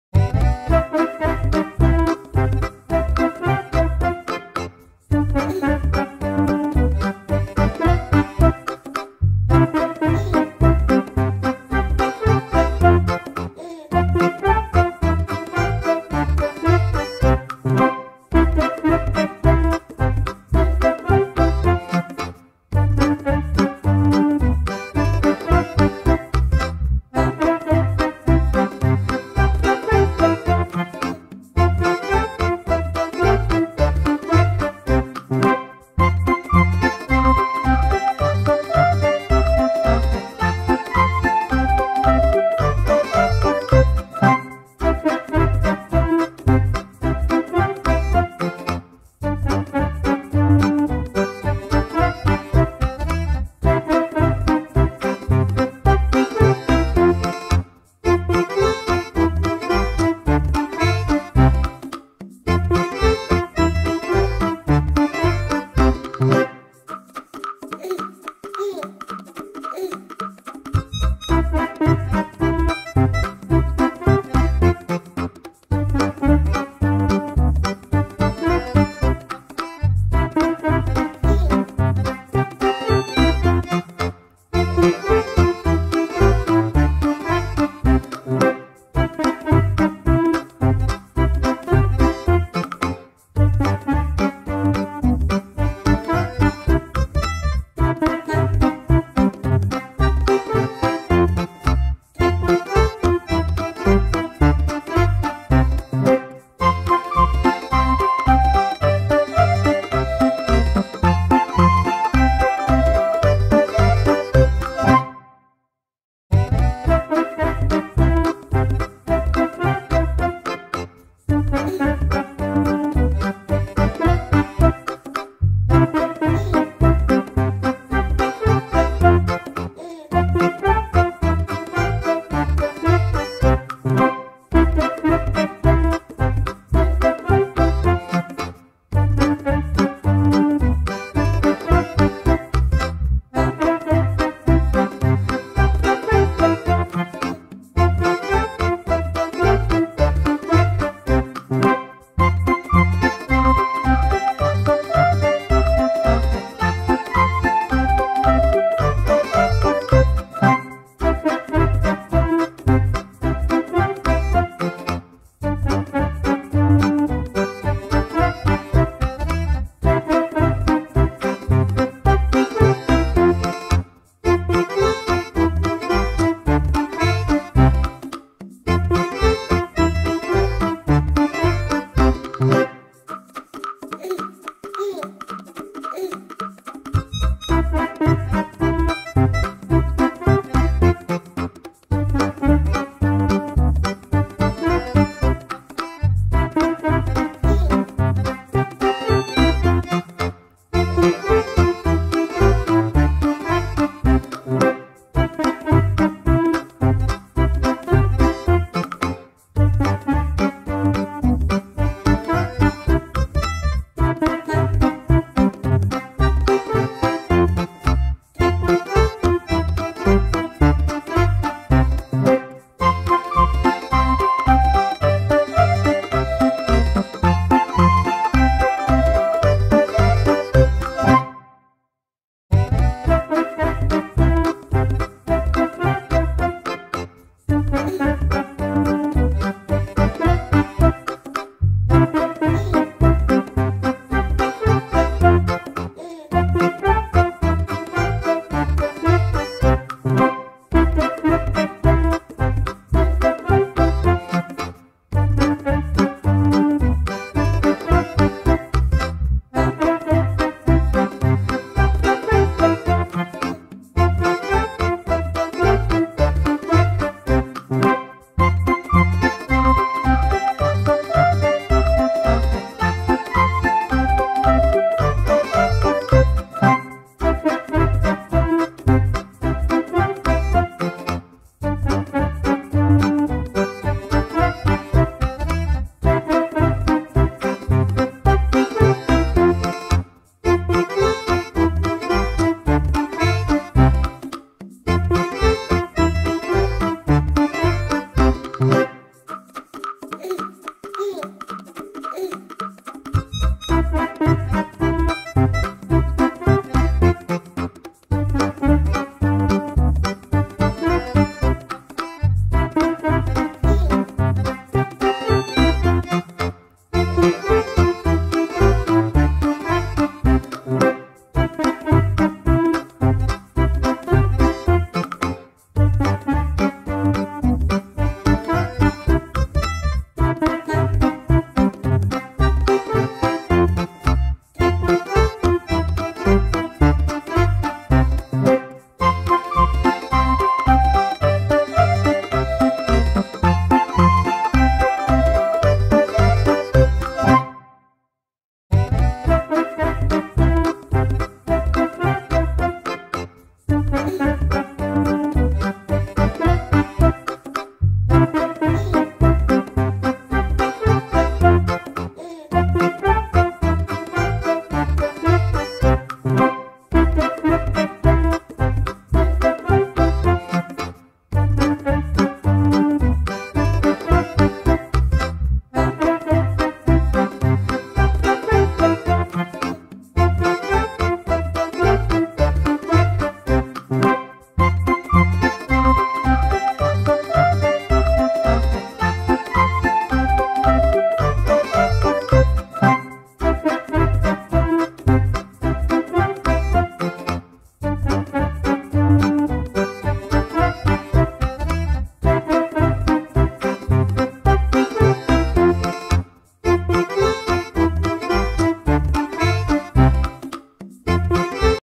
cute teddy bear step by step easy draw tutorial for toddler kids | nursery rhymes teddy drawing
step by step cute teddy bear drawing, easy teddy kids tutorial, preschool children teddy drawing with nursery rhymes,